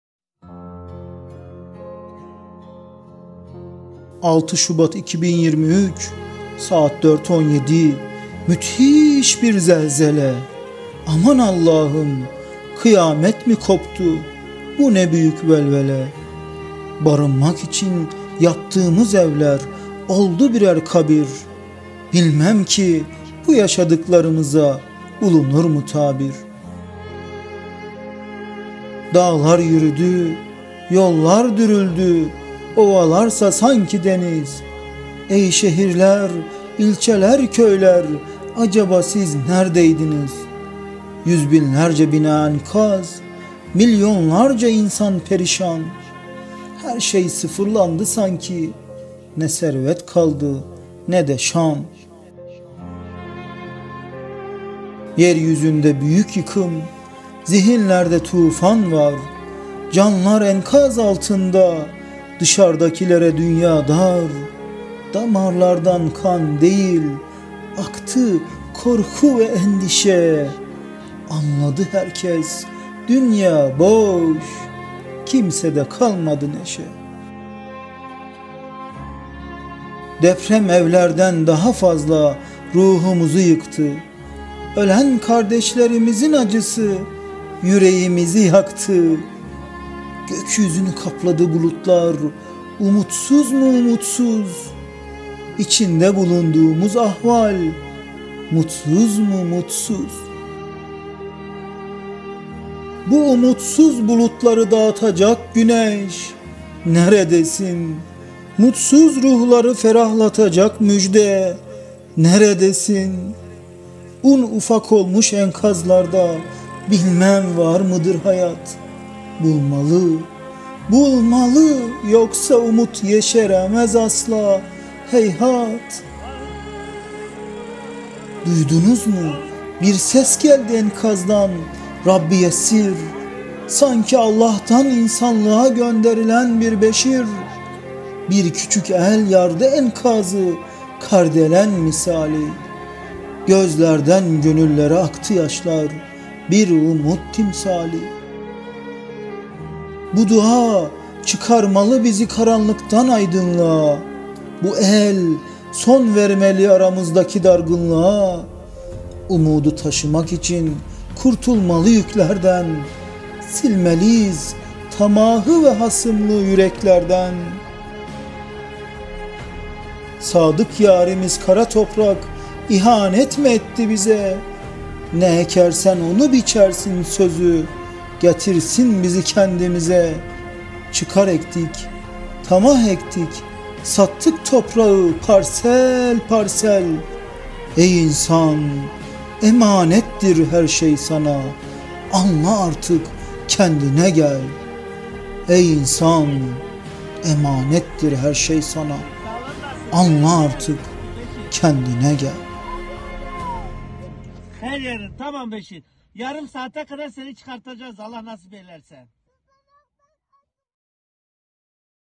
İsmail Pendik - Zelzeleden çıkan Beşir (Şiir)
Türkiye Cumhuriyeti tarihinin ve belki de dünya tarihinin en büyük afeti olan 6 Şubat 2023 tarihinde saat 04:17'de yaşadığımız deprem nedeniyle on binlerce canımızı kaybettik, milyonlarca insan olumsuz etkilendi ve yüzbinlerce bina hasar gördü. Tabir-i caizse 11 ilde kıyameti yaşadık. Bu şiire ilham olan hem bu acı durumdur hem de Diyarbakır Bağlar'da Hisami Apartmanın enkazından "Rabbi yessir..." duasını okuyarak kurtuldum diyen küçük Beşir'dir. Bu şiiri depremde hayatını kaybeden tüm kardeşlerimize,  bize yeniden umut olan Beşir gibi enkazlardan sağ kurtulan çocuklara ve depremden olumsuz etkilenmiş tüm insanlarımıza  ithaf ediyorum. Allah tüm vefat eden kardeşlerimize rahmet, yaralanan kardeşlerimize şifa ve hepimize sabr-ı cemil ihsan eylesin. Rabbim bir daha böyle afetler yaşatmasın.

Bu videoyu hazırlayan ve fon müziğinin seçiminde yardımcı olan Mehmet Ali Yaz ) kardeşime teşekkür ederim.

Şiir ve Yorum: İsmail PENDİK

Zelzeleden çıkan Beşir

6 Şubat 2023, saat 4.17... müthiş bir zelzele
Aman Allah'ım kıyamet mi koptu, bu ne büyük velvele!
Barınmak için yaptığımız evler oldu birer kabir
Bilmem ki bu yaşadıklarımıza bulunur mu tabir?

Dağlar yürüdü, yollar dürüldü, ovalarsa sanki deniz
Ey şehirler, ilçeler, köyler, acaba siz nerdeydiniz?
Yüzbinlerce bina enkaz, milyonlarca insan perişan
Herşey sıfırlandı sanki ne servet kaldı ne de şan

Yeryüzünde büyük yıkım, zihinlerde tufan var
Canlar enkaz altında, dışardakilere dünya dar
Damarlardan kan değil, aktı korku ve endişe
Anladı herkes dünya boş, kimsede kalmadı neşe

Deprem evlerden daha fazla ruhumuzu yıktı
Ölen kardeşlerimizin, acısı, yüreğimizi yaktı
Gökyüzünü kapladı bulutlar, umutsuz mu umutsuz
İçinde bulunduğumuz ahval, mutsuz mu mutsuz.

Bu umutsuz bulutları dağıtacak güneş, neredesin?
Mutsuz ruhları ferahlatacak müjde, neredesin?
Un ufak olmuş enkazlarda bilmem var mıdır hayat?
Bulmalı, yoksa umut yeşermez asla , heyhat!

Duydunuz mu? Bir ses geldi enkazdan,"Rabbi yessir”
Sanki Allah'tan insanlığa gönderilen bir Beşir.
Bir küçük el yardı enkazı, kardelen misali
Gözlerden gönüllere aktı yaşlar, bir umut timsali.

Bu dua çıkarmalı bizi karanlıktan aydınlığa
Bu el, son vermeli aramızdaki dargınlığa
Umudu taşımak için kurtulmalı yüklerden
Silmeliyiz tamahı ve hasımlığı yüreklerden.

"Sadık yarimiz kara toprak" ihanet mi etti bize?
"Ne ekersen onu biçersin" sözü getirsin bizi kendimize
Çıkar ektik, tamah ektik, sattık toprağı parsel parsel
Ey insan! Emanettir her şey sana, anla artık kendine gel!